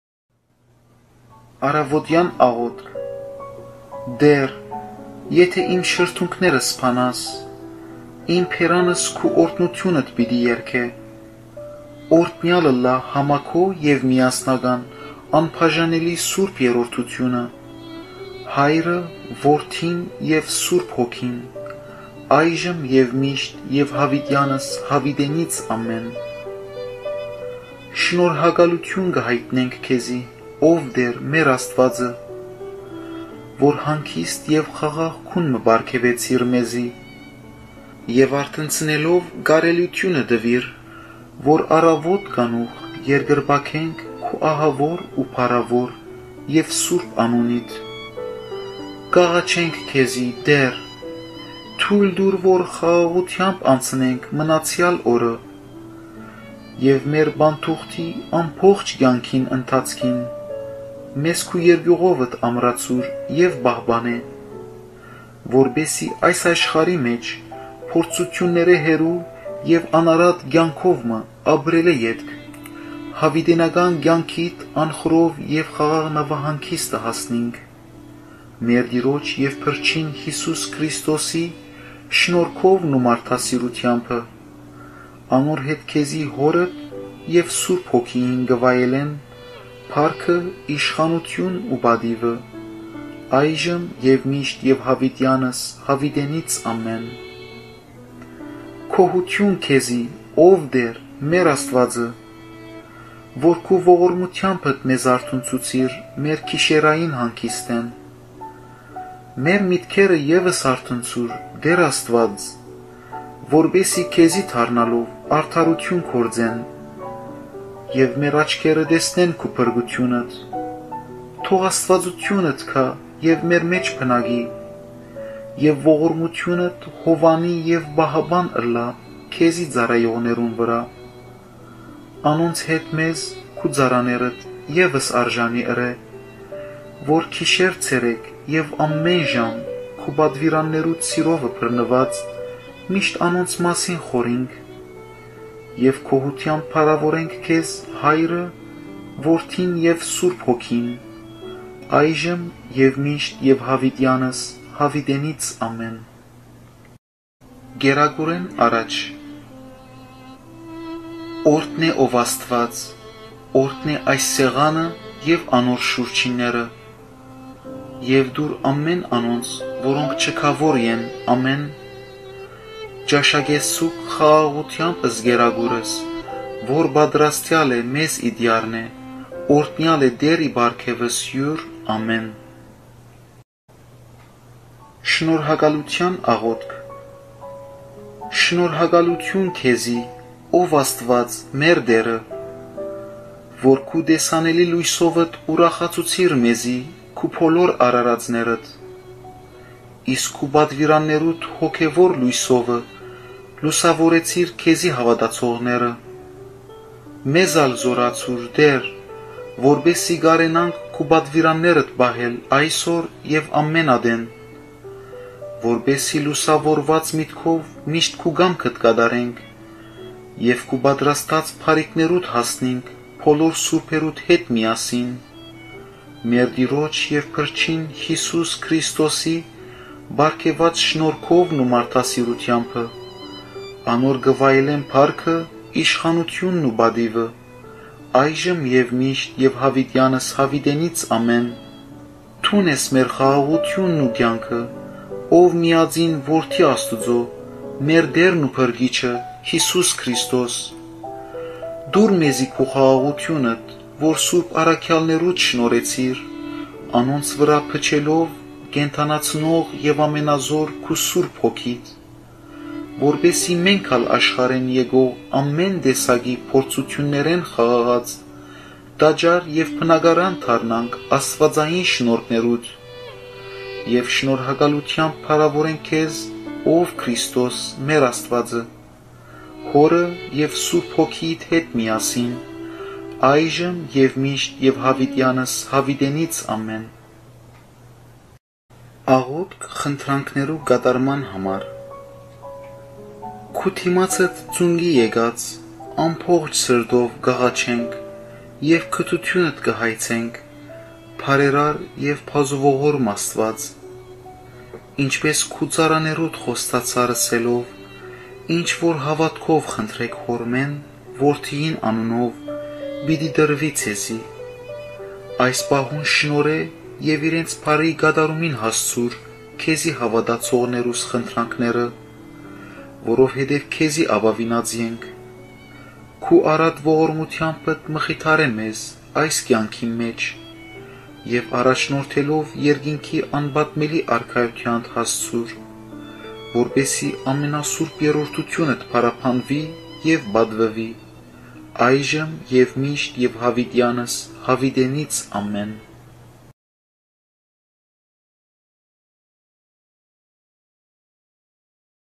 Prieres Arménienne ╬ ՅԻՍՈՒՍԱԲՈՅՐ ԱՂՕԹՔՆԵՐ ♥ 1
Հայր Պարթև Կիւլիւմեանի մեկնաբանութեամբ՝

1-Առաւօտեան Աղօթք
2-Աղօթք ճաշէն առաջ
3-Շնորհակալութեան Աղօթք
4-Խնդրանքներու Կատարման Աղօթք